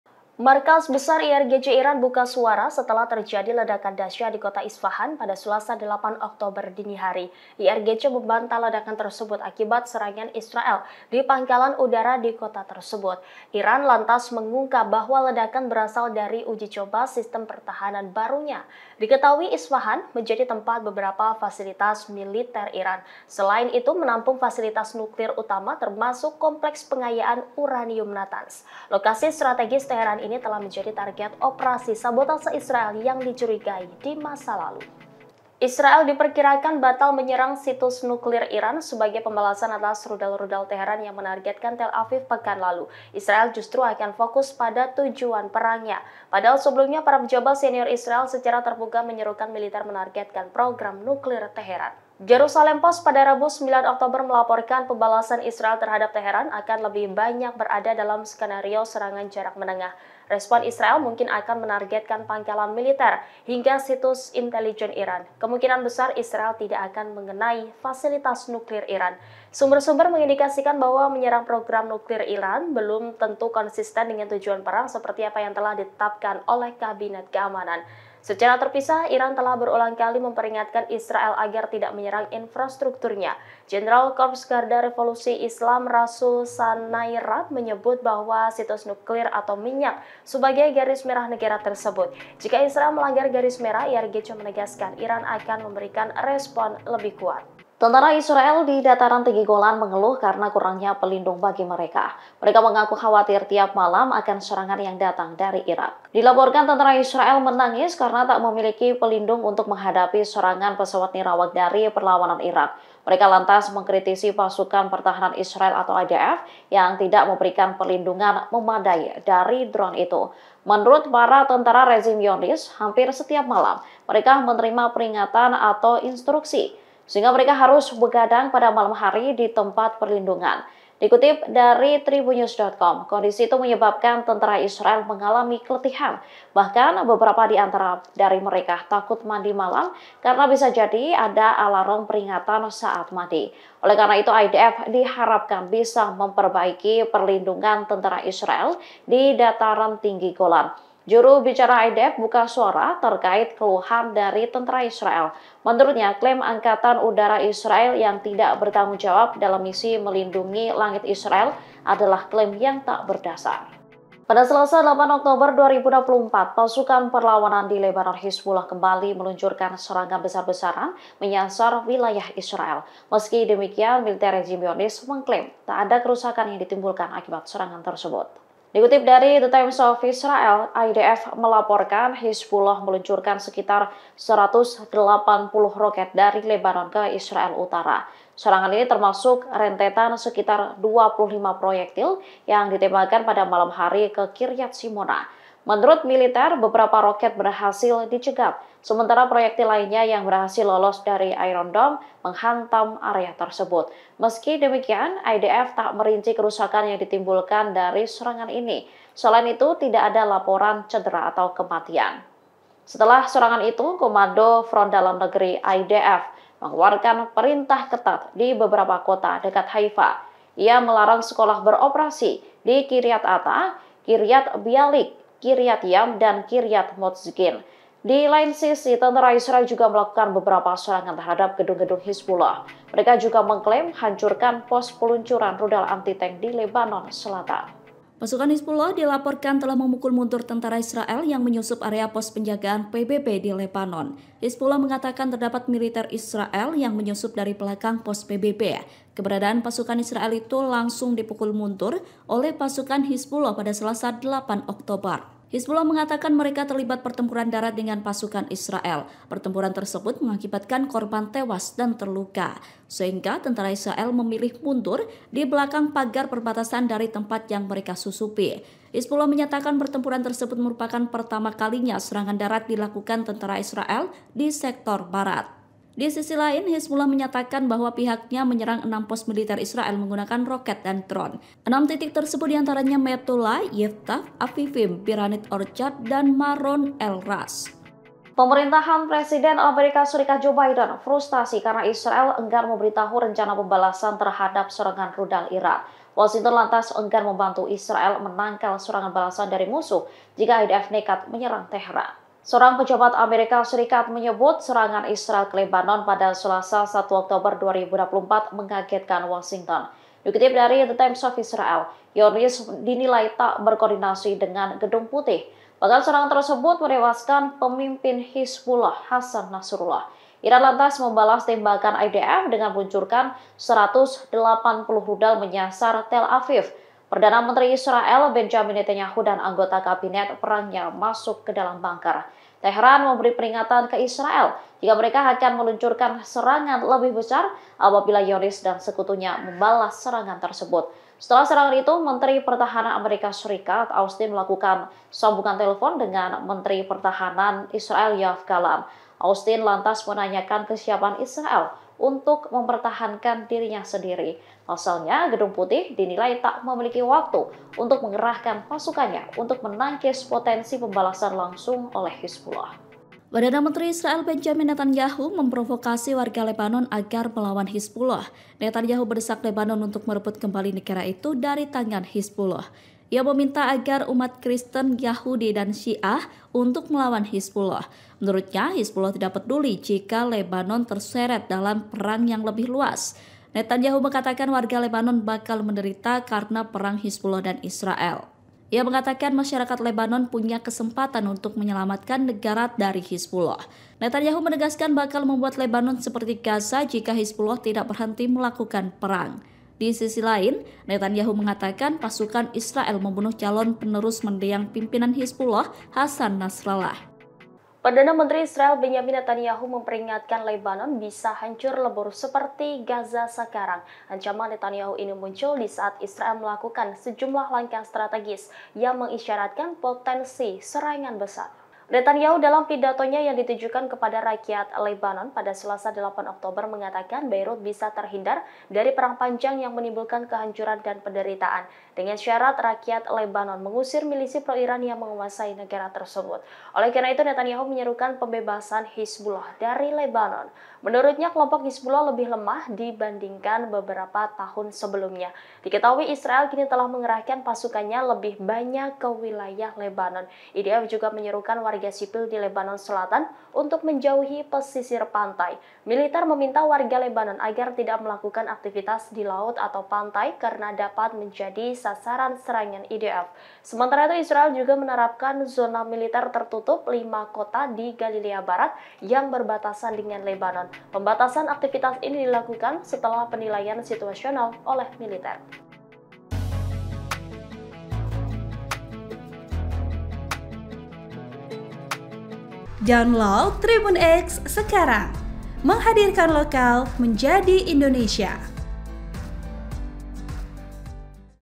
Rangkuman Ke-369: Isu Israel Batal Serang Situs Nuklir Iran, Netanyahu Sebut Lebanon Bisa Hancur
TRIBUN-VIDEO.COM - Berikut sejumlah berita terkait perang Israel dan Hamas yang terangkum dalam Tribun Video:

1. Markas Besar IRGC Buka Suara Seusai Ledakan di Isfahan, Berasal Uji Coba Sistem Pertahanan Baru

2. Israel Diperkirakan Batal Serang Situs Nuklir Iran, Targetkan Situs Intelijen & Pangkalan Militer

3. Tentara Israel Menangis Takut Diserang Drone Irak, Harus Begadang & Waspada di Malam Hari

4. 25 Roket Hizbullah Turut Menghantam Kiryat Shmona, Beberapa Proyektil Berhasil Dihadang Iron Dome

5. Tentara IDF Kelimpungan Dipukul Mundur Hizbullah saat nekat Menyusup Pos PBB di Lebanon

6. AS Enggan Bantu Israel jika IDF Nekat Serang Teheran, Joe Biden Frustasi Zionis Tak Berkoordinasi

7. Netanyahu Memprovokasi Warga Lebanon, Minta Umat Kristen, Yahudi dan syiah Melawan Hizbullah

8. Netanyahu Sebut Lebanon Bisa Hancur Lebur seperti Gaza, Israel Isyaratkan Potensi Serangan Besar

Nama Program: Tribun Video Update
Editor Video: Ananda Bayu S
Uploader: bagus gema praditiya sukirman